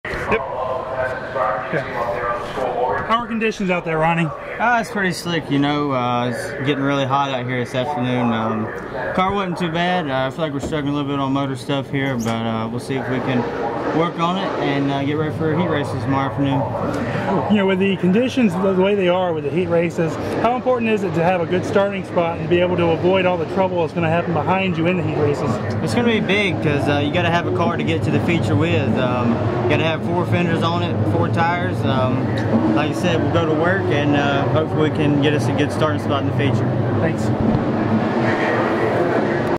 Martinsville 2013 - Ronnie Bassett, Jr. Post-Qualifying Interview
Ronnie Bassett, Jr., talked with Matt Weaver about his qualifying run at Martinsville on Saturday.